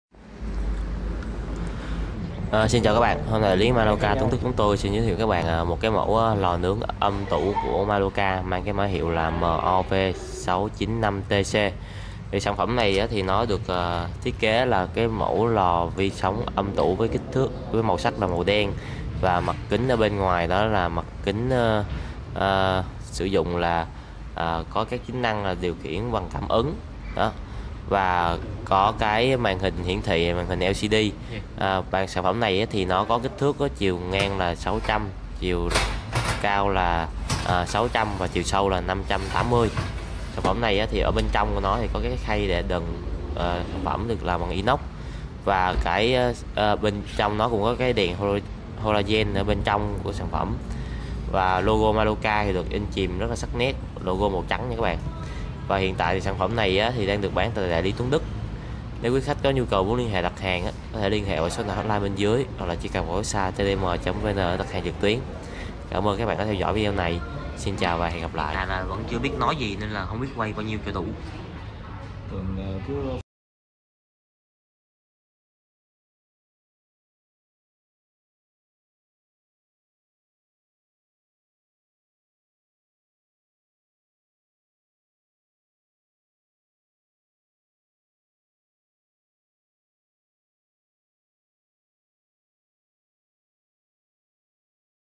TDM.VN | Review lò nướng điện Malloca MOV-659TC âm tủ đa chức năng chính hãng
Thông tin cơ bản lò nướng âm tủ Malloca MOV-659TC :
Dung tích : 70L
Điện áp : 220-240V/50Hz
Công suất: 3000W
Kích thước: W595 x D575 x H595mm
Xuất xứ : Châu Âu
Bảo hành : 3 năm
Tính năng lò nướng điện âm tủ MOV-659TC Malloca  :
Mặt kính màu đen
9 chức năng
Hệ thống điều khiển cảm ứng
Màn hình hiển thị LCD
Chức năng hẹn giờ
Đèn halogen bên trong lò
Chức năng nướng xiên quay
Quạt tản nhiệt
Quạt tuần hoàn
Cửa lò đóng mở giảm chấn
Khóa phím an toàn trẻ em